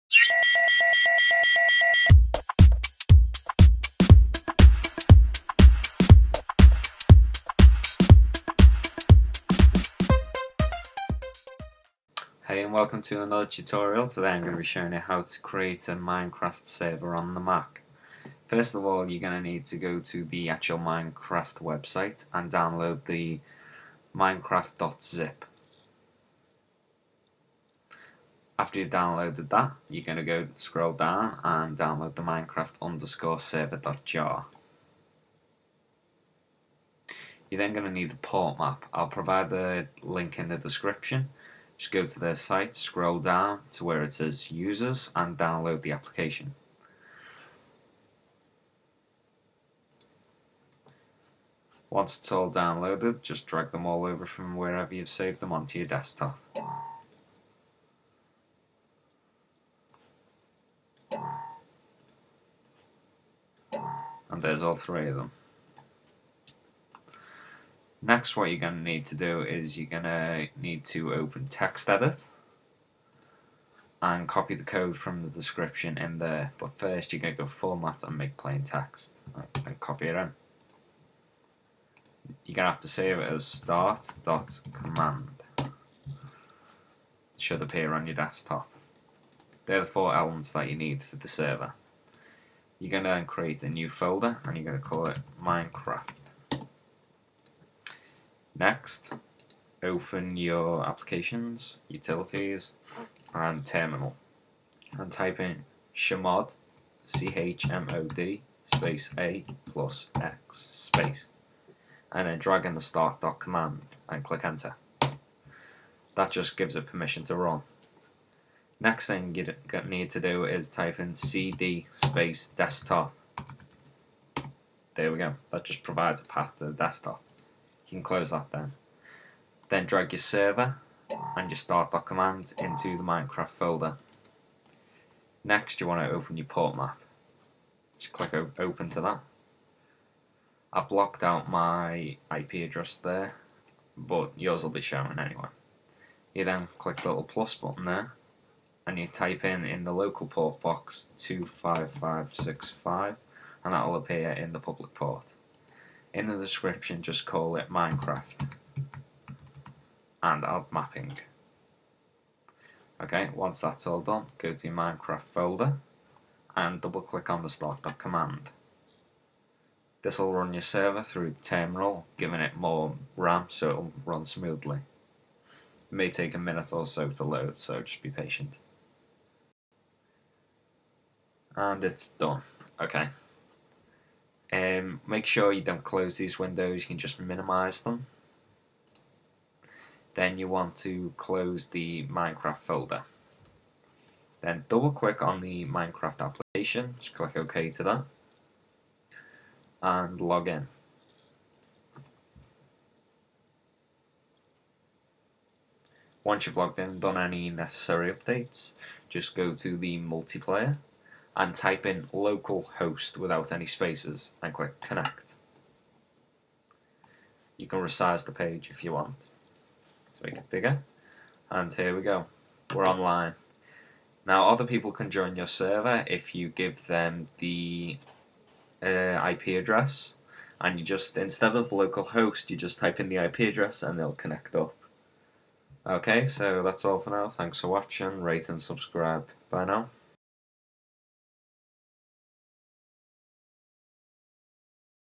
How to Set up a MineCraft Server for Mac
In this video I show you how to set up a MineCraft server on your Personal Computer. This video is for a Mac operating system; for windows you simply just download the server file from MineCraft.net and you're good to go.
1) Code to put in textedit:

#!/bin/bash
cd "`dirname "$0"`"
java -Xmx1G -Xms1G -jar minecraft_server.jar
2) Code for terminal:

Chmod a+x Make sure you leave a space then drag the file in

[For Mac users you want the Mac OSX 'Minecraft.zip' game and the server at the bottom of the page with the extension '.JAR']